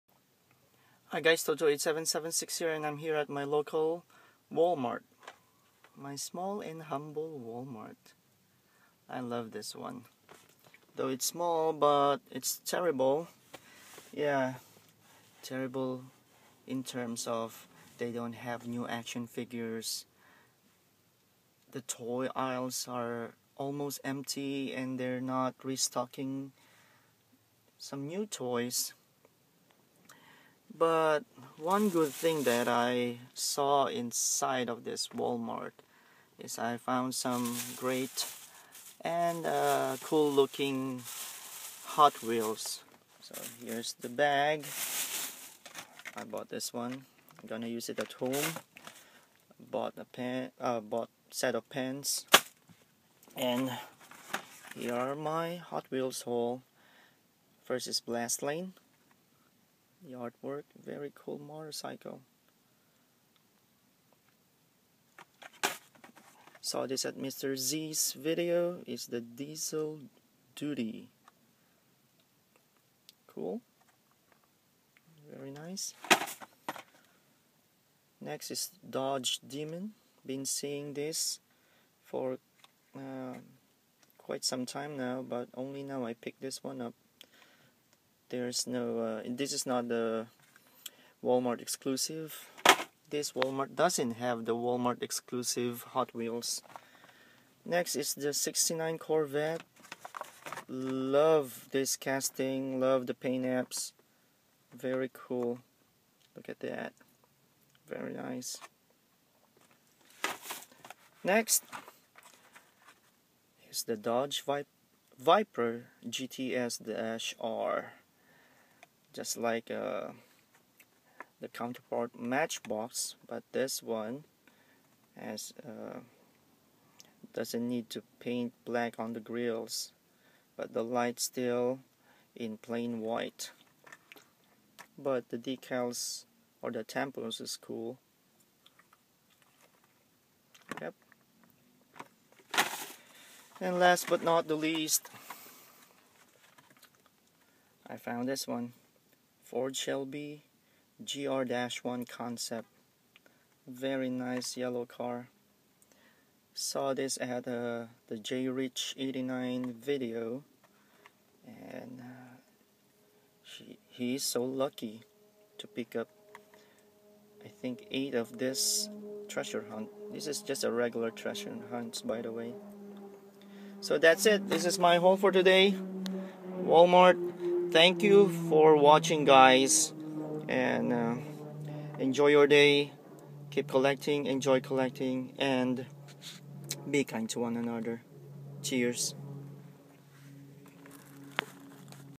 Friday hot wheels haul
Got them at my small & humble Walmart. Can't shoot them inside cos a lot of people shopping.

Please continue to support our super friends :)